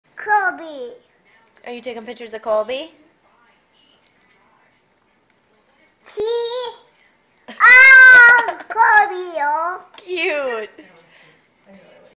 Self pictures
Colby taking pictures of his favorite thing...himself!